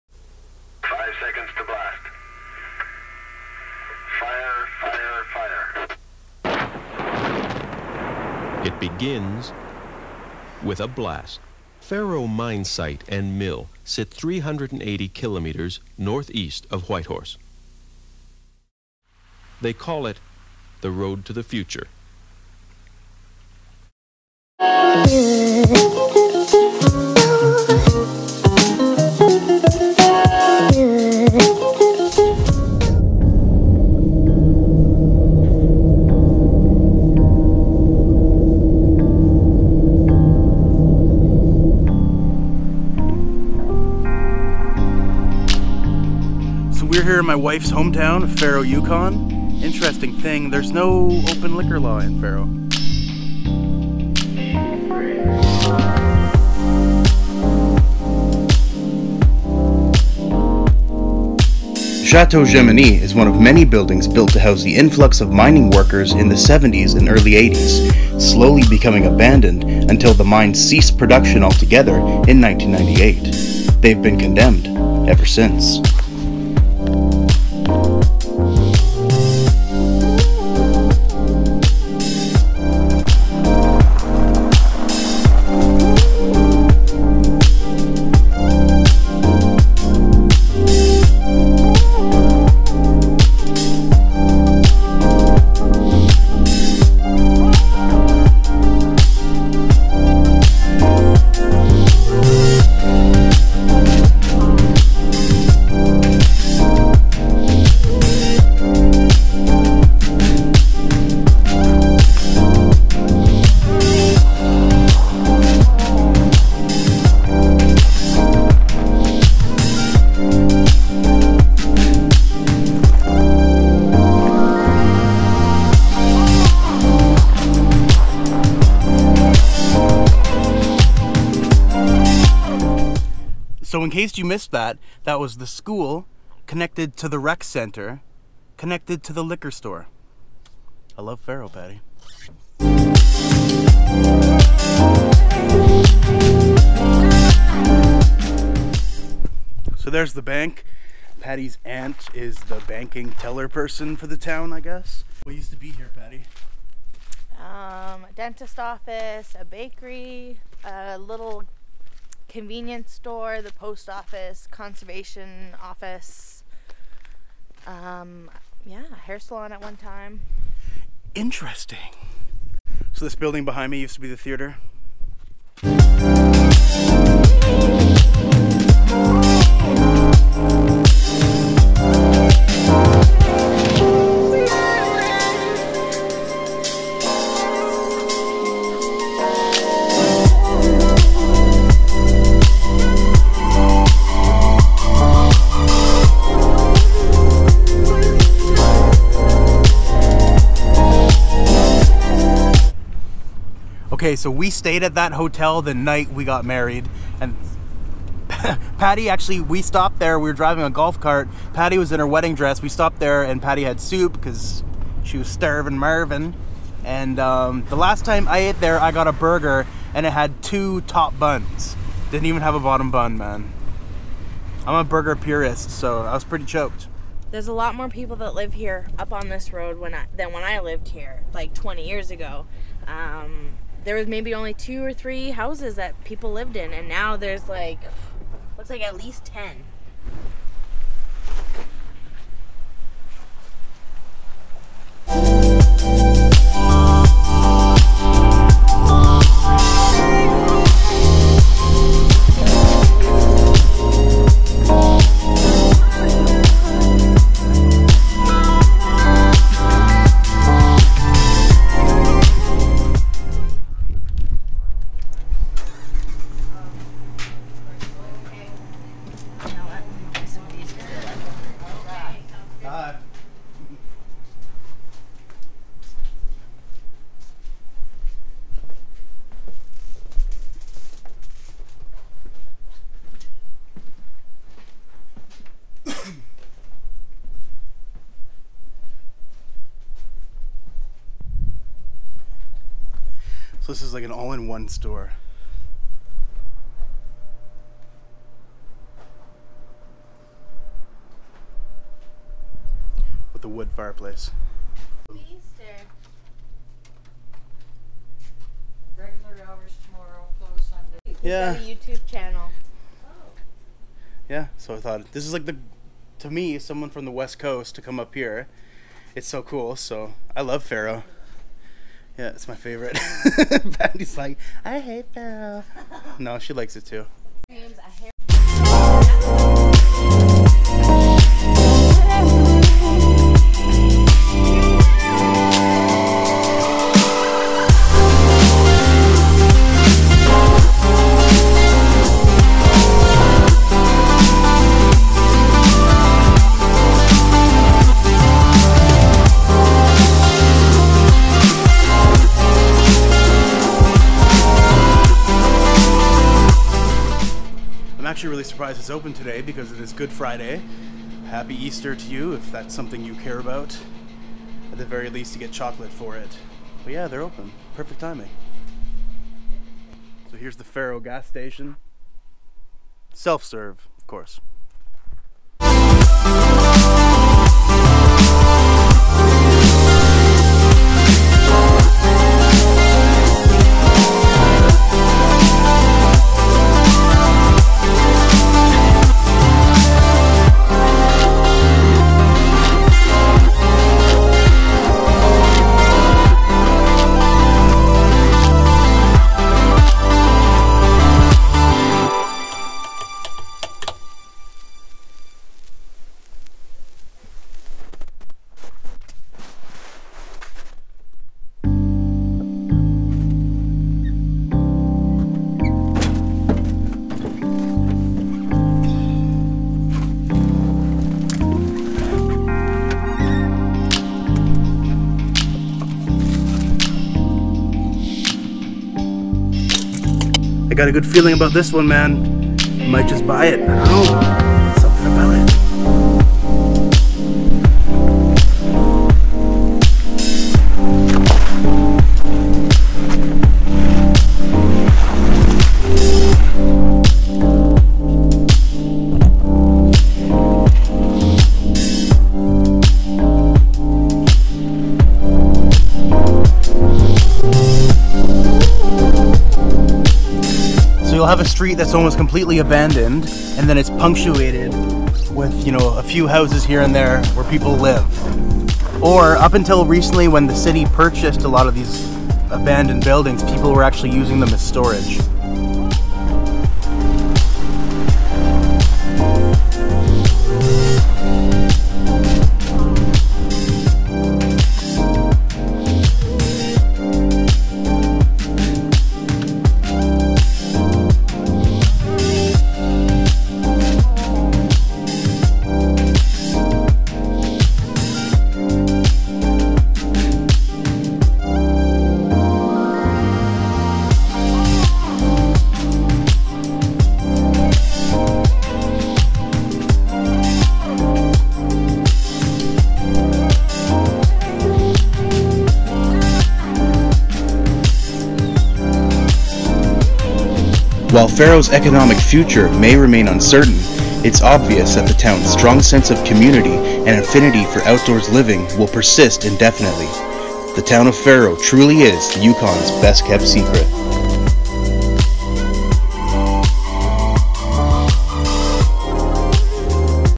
Abandoned Ghost Town? || Faro Yukon
Explore the town of Faro, the Yukon’s best kept secret. The fact that half the town is abandoned does nothing to dampen the sense of community.